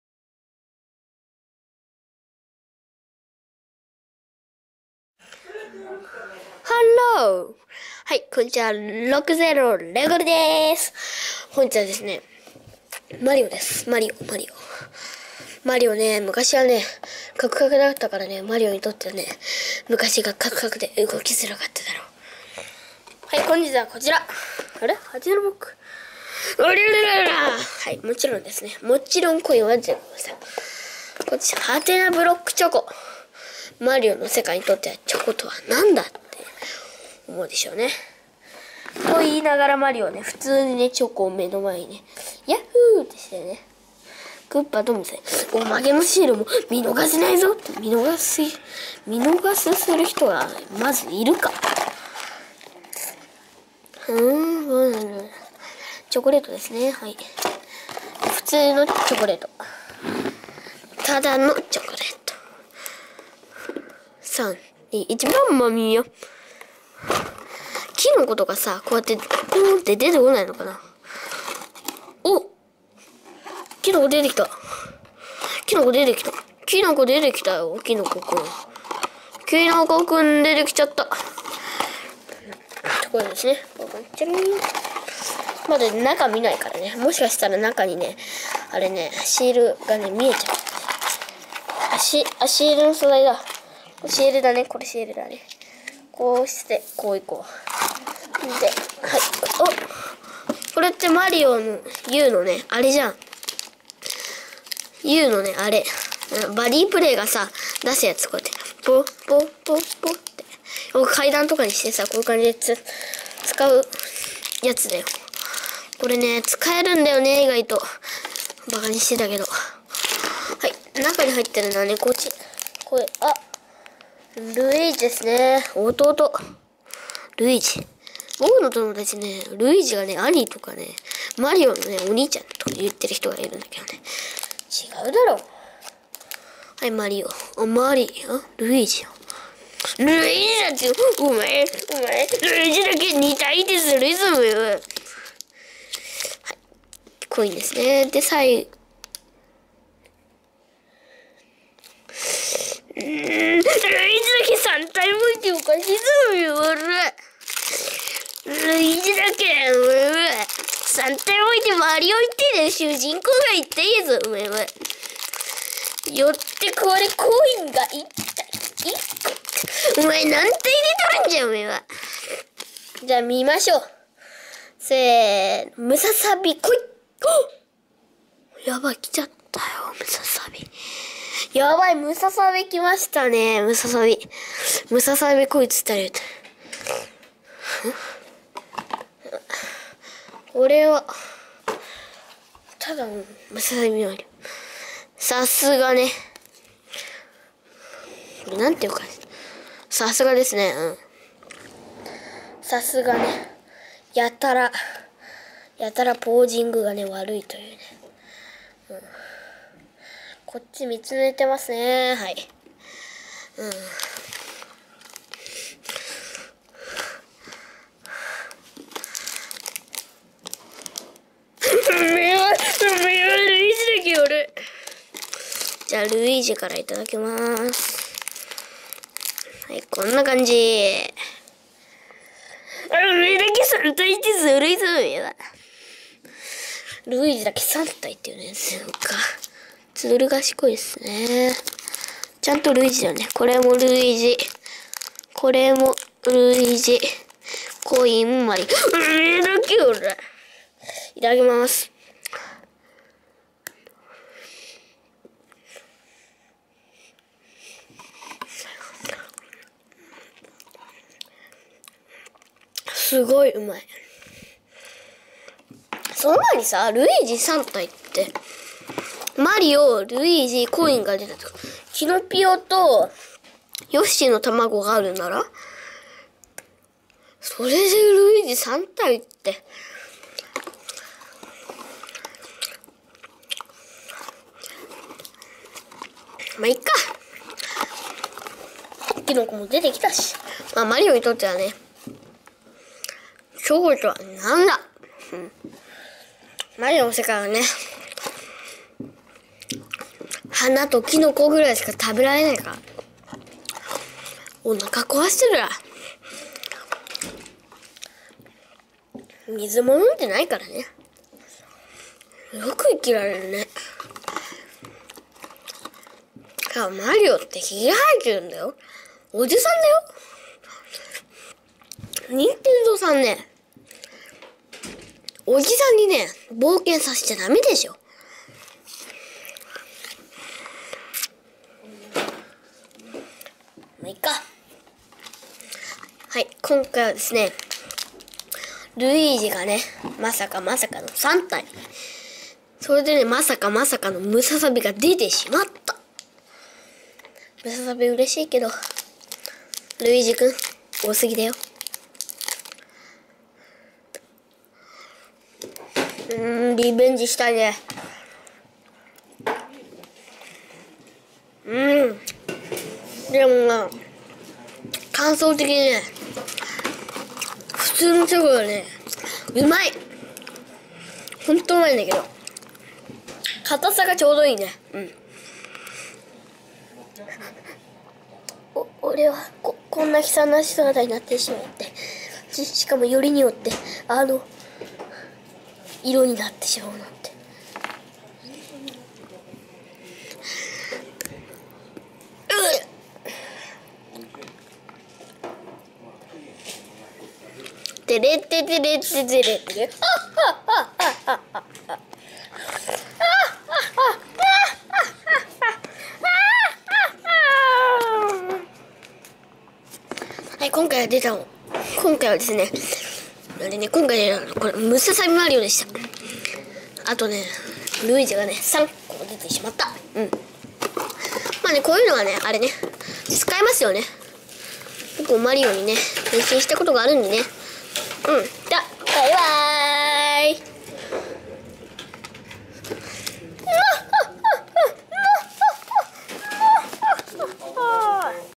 ★ハテナブロックチョコ★でた！
箱がかわいいハテナブロックチョコ、、おまけシールはなにかな！？